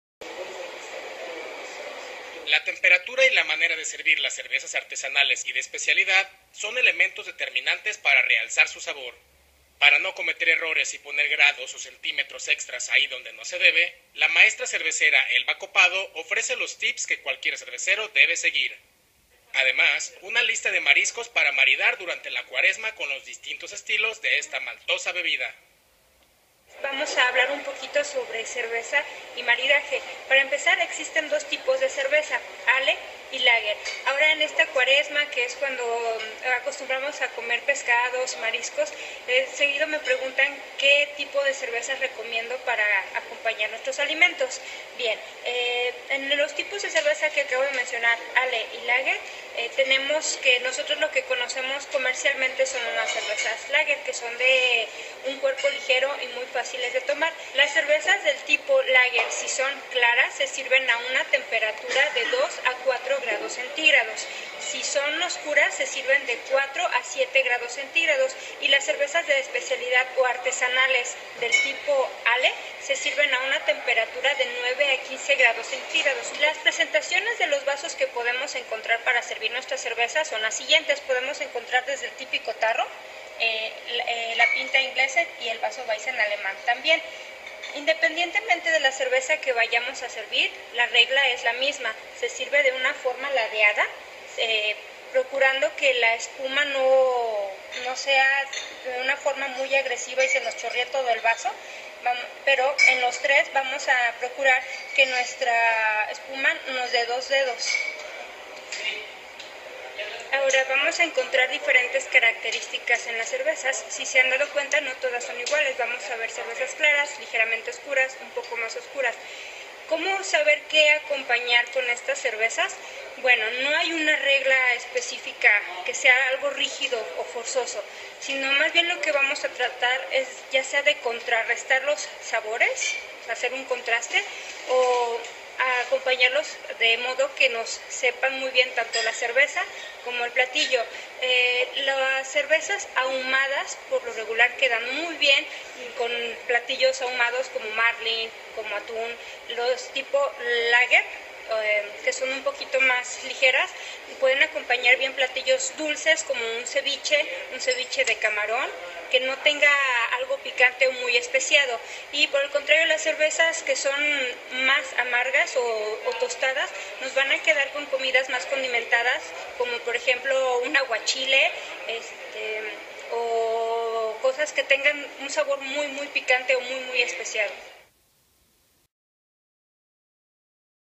Cerveza y maridaje.
Una entrevista para el periódico mural.
Si deseas conocer mi nueva ubicación y tomarte unas cervezas, estoy en la ciudad de Guadalajara, ésta es mi página de facebook